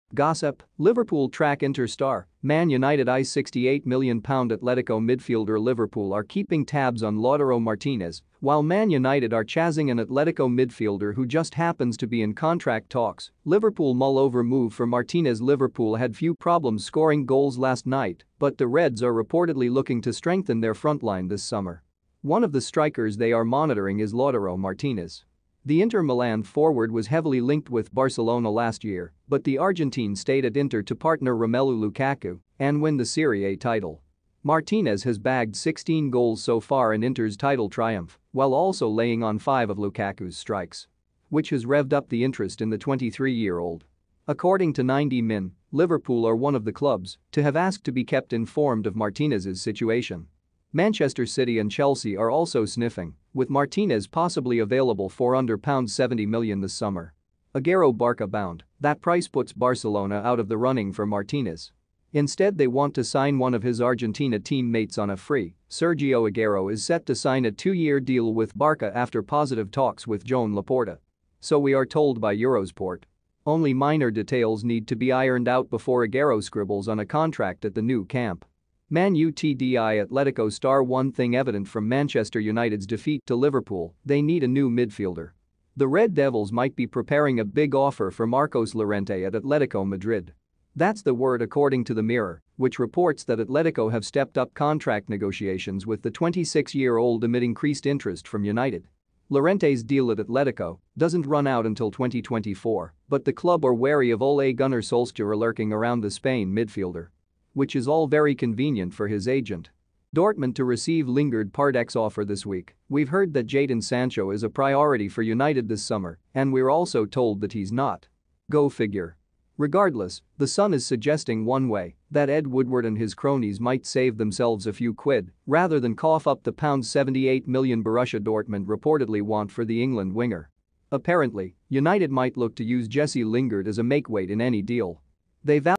Gossip: Liverpool track Inter star, Man Utd eye £68m Atletico midfield
Gossip: Liverpool track Inter star, Man Utd eye £68m Atletico midfielder